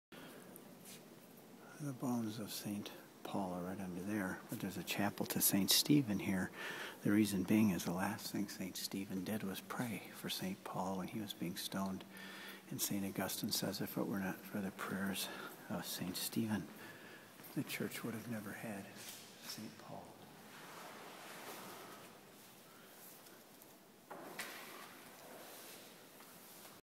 video19.mov: 2011-05-06 Martyrdom of Apostles
Whew! Old record 14 miles in Israel; new record today in Rome of 16.3 miles! Started outside Rome at the Catacombs and ran back to St. Peter's and all the way to our Ergife Hotel. I specifically followed the last days of the Prince of the Apostles. I document their martyrdoms and the place of their bones.